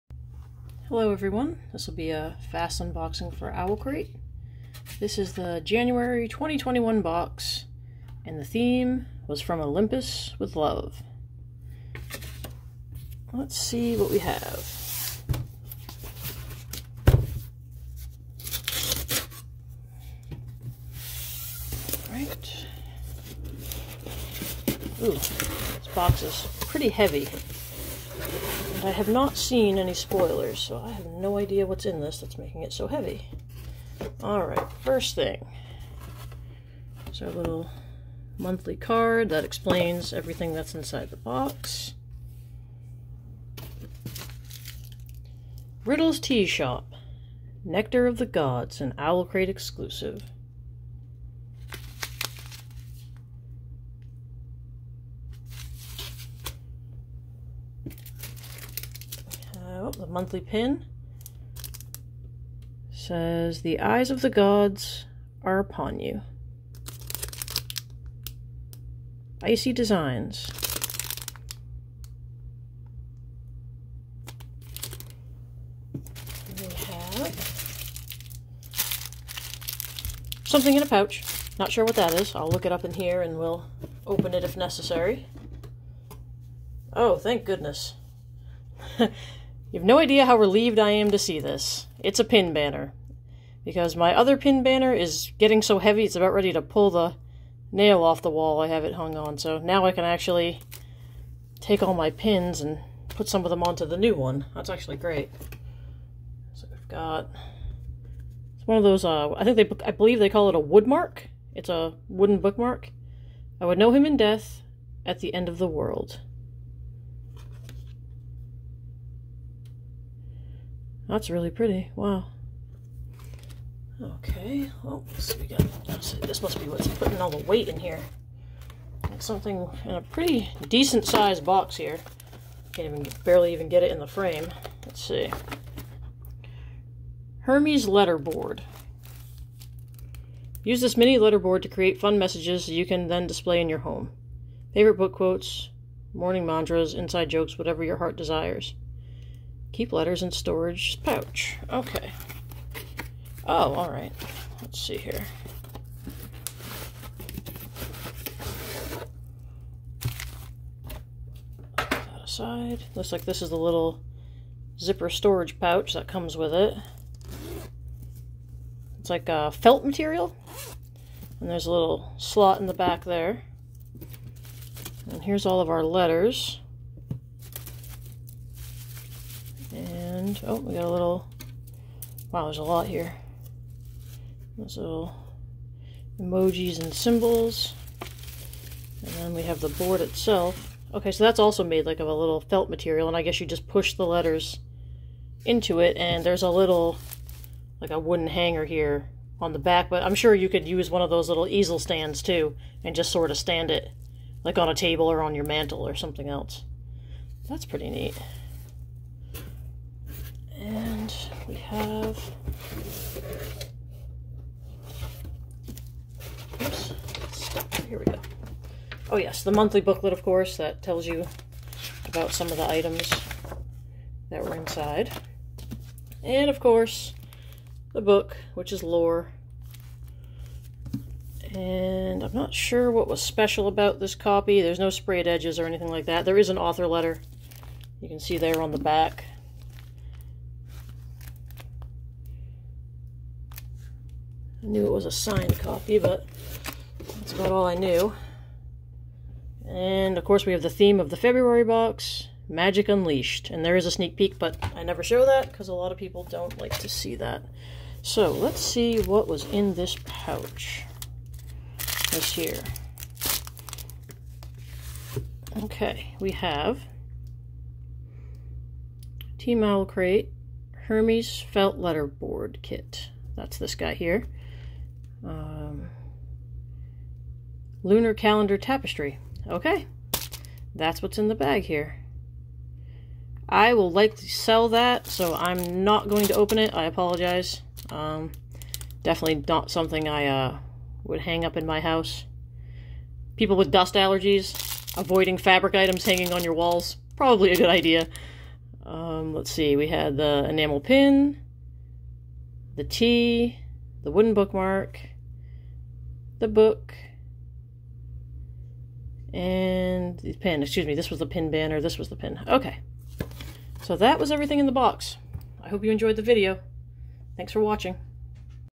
Owlcrate January 2021 From Olympus With Love fast unboxing
A fast unboxing of the Owlcrate January 2021 box. The theme was From Olympus With Love.

This is what was included in the box:
1. Felt Letter Board Kit - created by Team OwlCrate
2. Lunar Calendar Tapestry - designed by Eden Cooke
3. Olive Branches pin banner - designed by Michelle Gray
4. Nectar of the Gods loose leaf tea - created by Riddle's Tea Shop
5. Wooden bookmark - designed by In The Reads inspired by The Song of Achilles and Circe.
6. Book - Lore by Alexandra Bracken. This edition has an exclusive cover, an author letter, and it's signed.
7. Medusa shield enamel pin - inspired by Lore and created by Icey Designs.

Overall I really liked the items in this box. My current pin banner is too full so I was happy one was included in this box. The changes to the book were a bit underwhelming. The cover wasn't all that different. Owlcrate is falling behind Fairyloot and Illumicrate on the special features on their books.